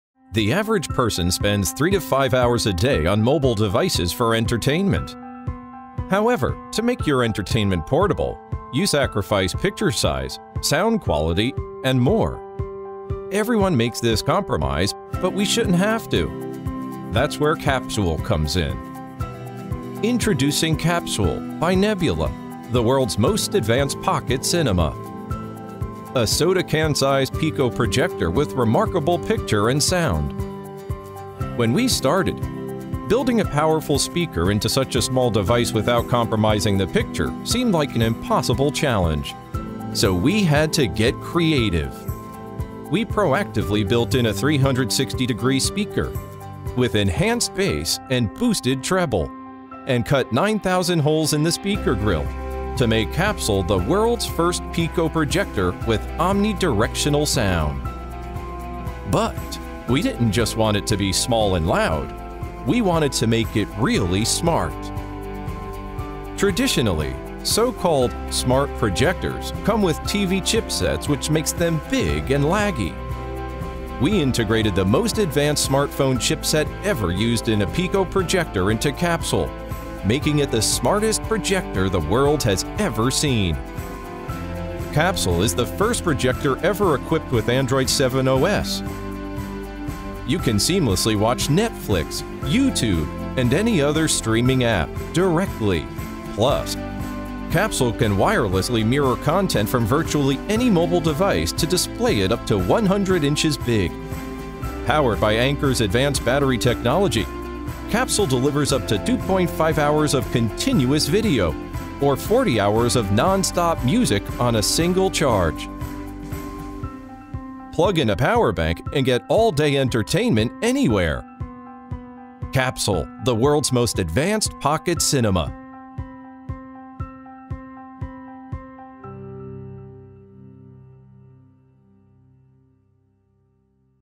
Capsule - The World’s Most Advanced Pocket Cinema
A soda-can size smart projector with high-contrast, a 360° speaker, Android 7 and extended playtime.Nebula Capsule is an immersive cinema that you can enjoy anytime, anywhere. Using Android 7.0 it plays content from your favorite video streaming app or mirrors your phone’s screen to create a remarkably detailed picture up to 100 inches.
Complementing the stunning image quality is a class-leading 360° speaker, and 2.5hr battery life. All in a body no larger than a soda can. Portable entertainment will never be the same again.